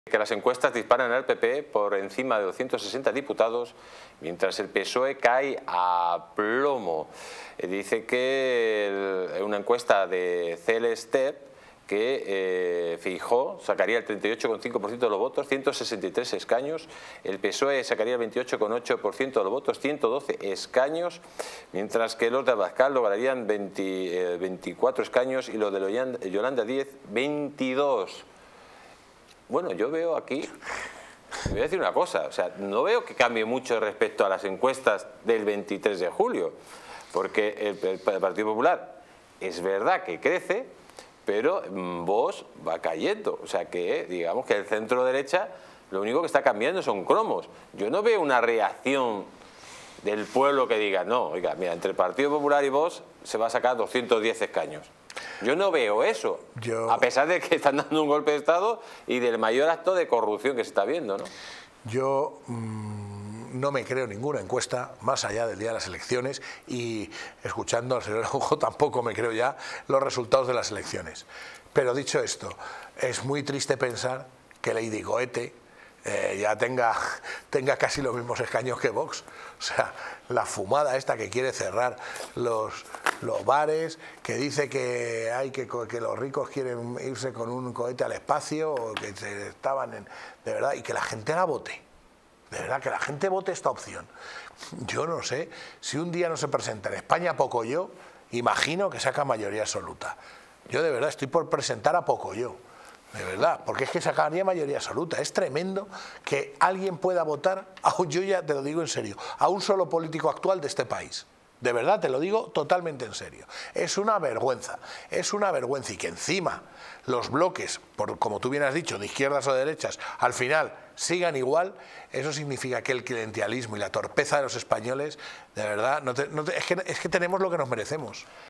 🔴Encuestas disparan PP por encima de 160 diputados, mientras el PSOE cae a plomo por la trama Ábalos
🔴 Las últimas encuestas muestran que el PP supera los 160 escaños, mientras que el PSOE cae debido al escándalo de Ábalos. ¡Sintonice Distritotv para conocer las últimas actualizaciones y análisis políticos!